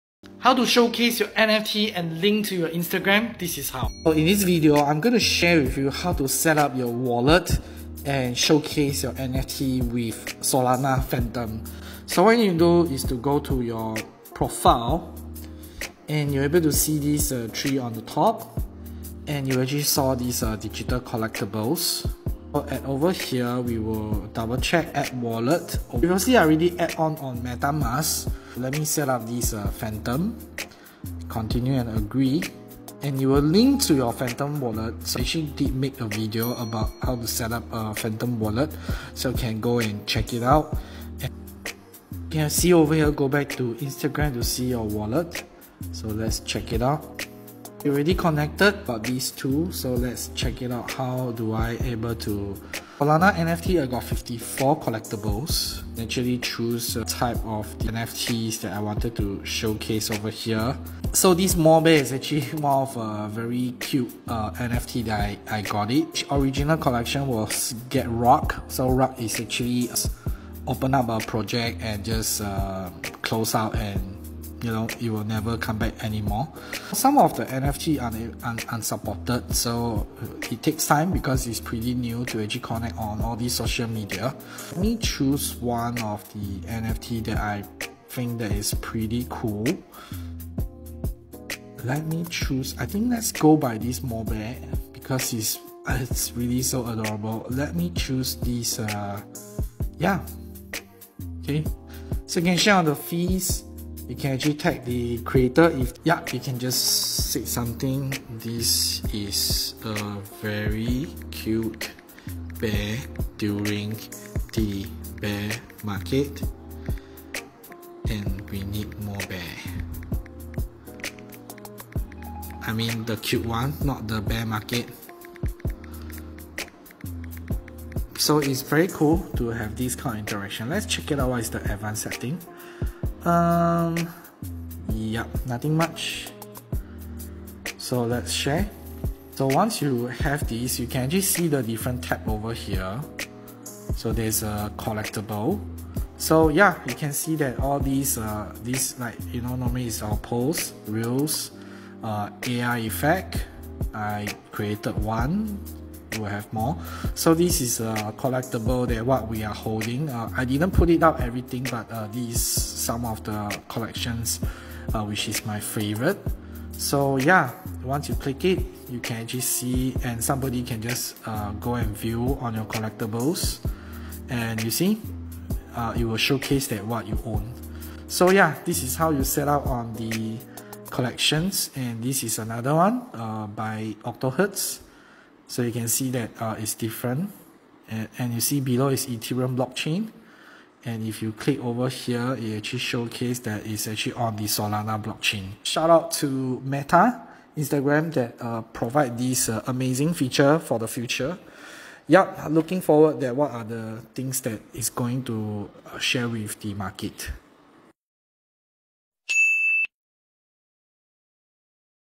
How to connect Solana NFT to your Instagram 2022
how to connect NFT to your Instagram.
solana nft on Instagram.
how to connect phantom wallet to Instagram.